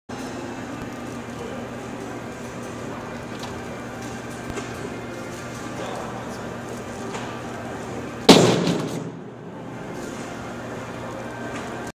YETI Snow MX - Bike Binderz Demo 2
Here is a great way to secure your SNOWMX bike in your truck or trailer.

Built in Canada by BIKE BINDERZ, these heavy duty, but easy to use bike tie downs lock over your foot pegs with a quick pin, and a cantilever adjuster.

Just put the BIKE BINDERZ over your foot peg, install the quick pin, push down on the cantilever and your bike is locked down.

Comes with the 12” of floor track to mount to your truck box or trailer floor.

Tested to 1500lbs of force.

Great product to eliminate the tie down tangle up with multiple bikes and way stronger, worry free and awesome in multiple colors.